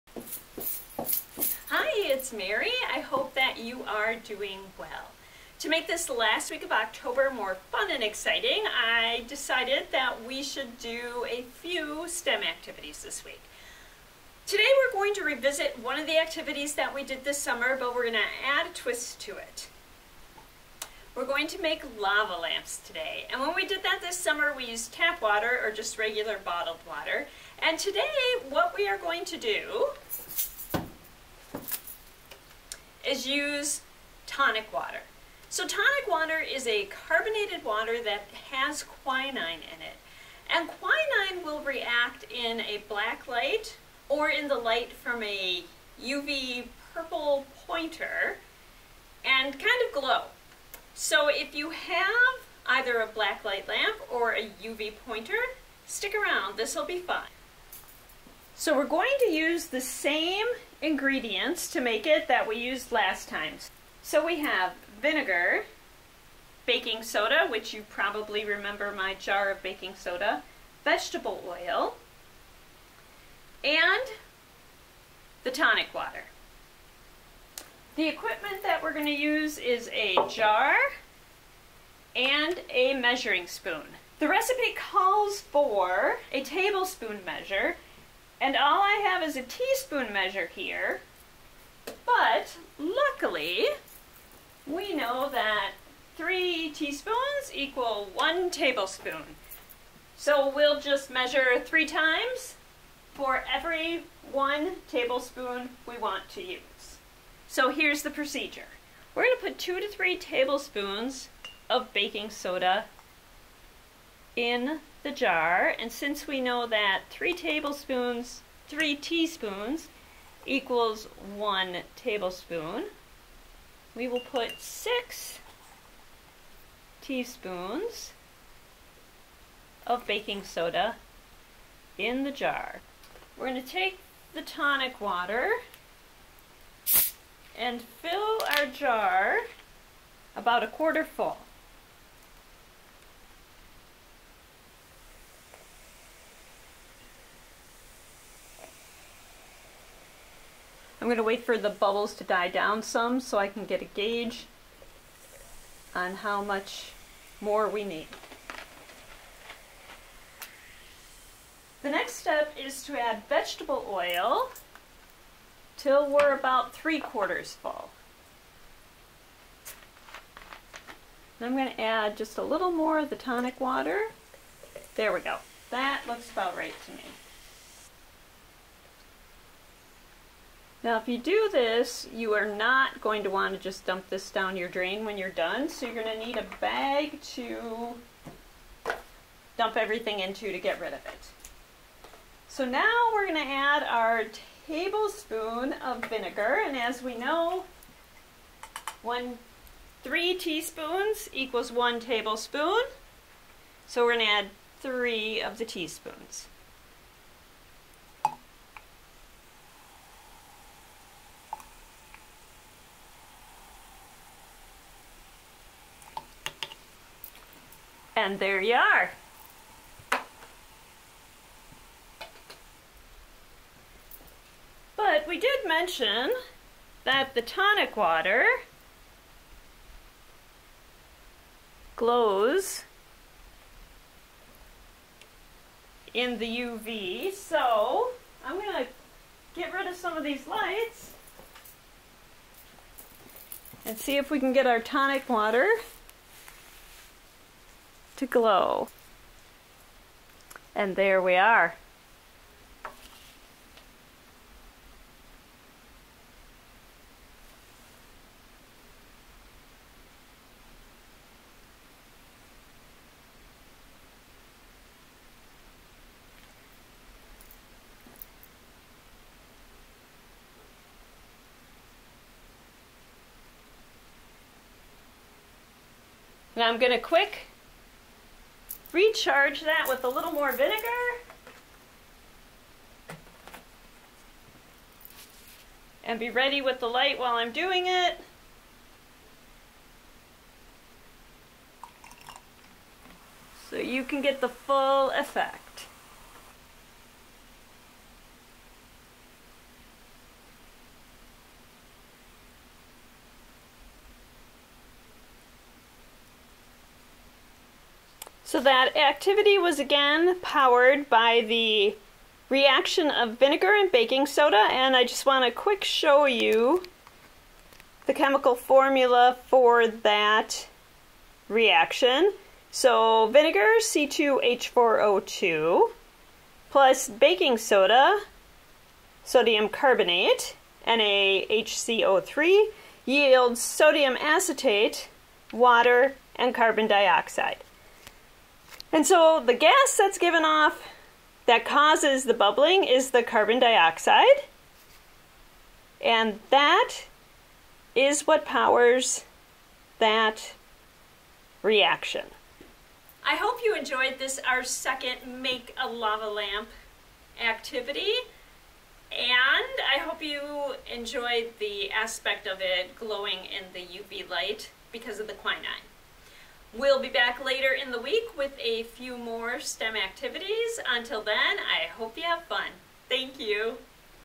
Explore STEM: Make a Glowing Lava Lamp
Just in time for October 31st, we'll revisit making a lava lamp!  This time it will glow in black light or with a UV purple light pointer!